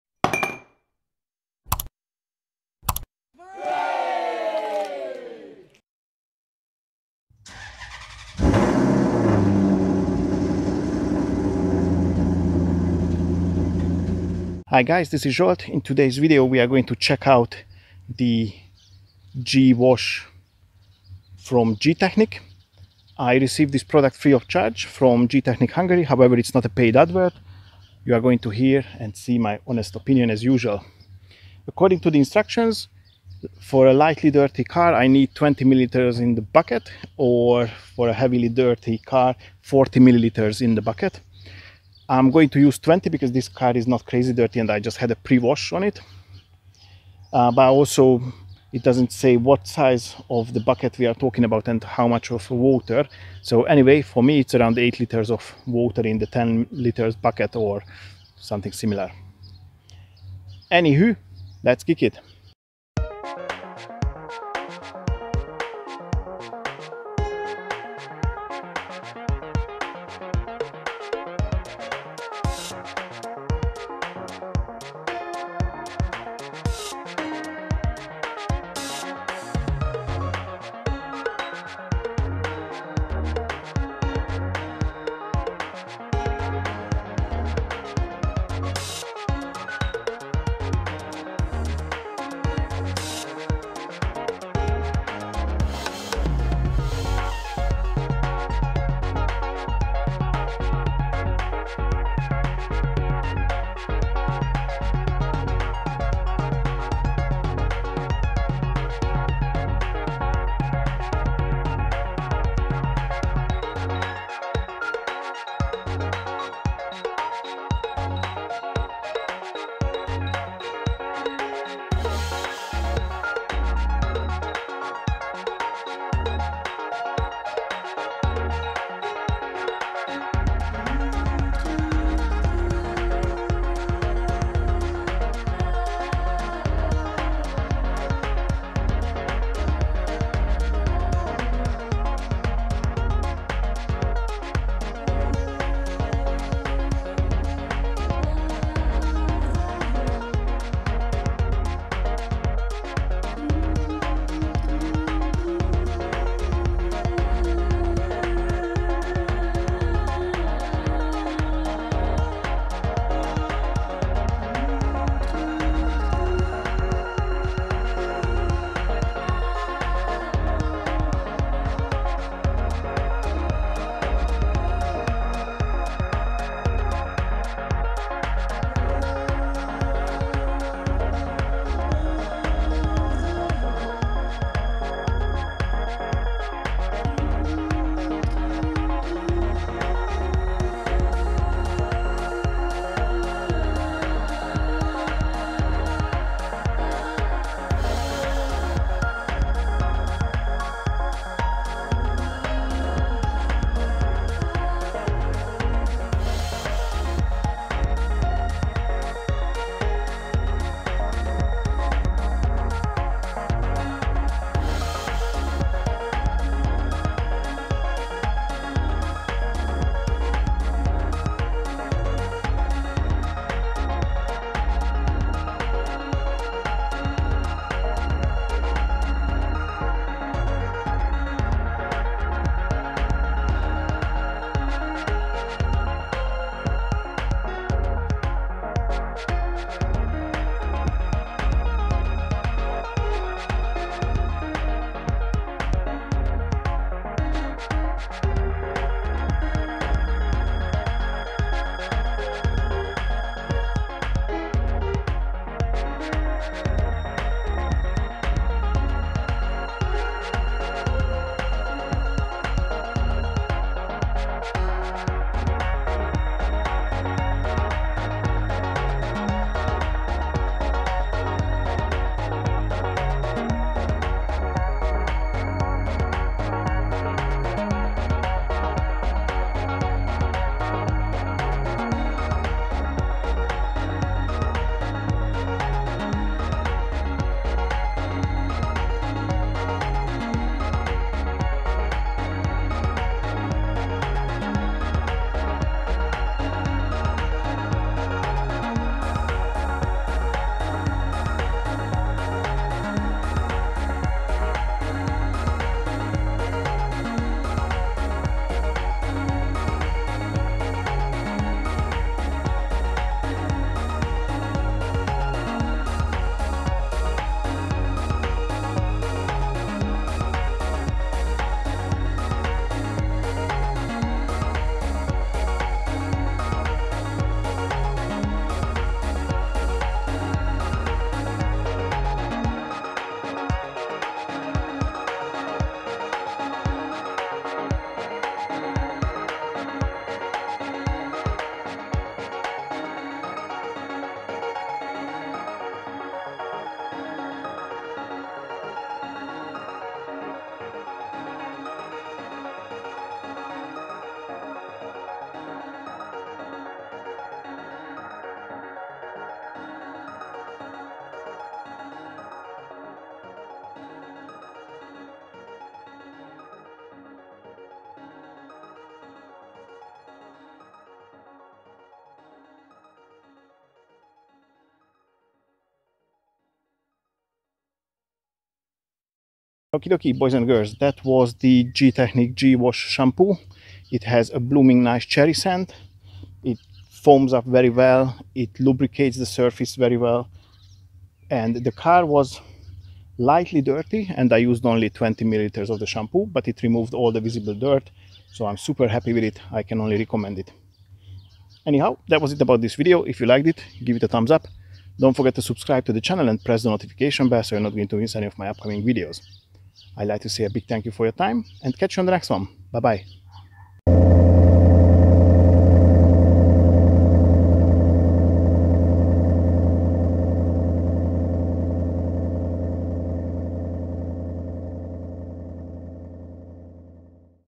Gtechniq G-Wash deep cleaning shampoo test
This video is not a paid advert, however I received the product free of charge from Gtechniq Hungary, in order to be able to show you. It is about the Gtechniq G-Wash deep cleaning shampoo's test.

Sonax car care products: sonax.hu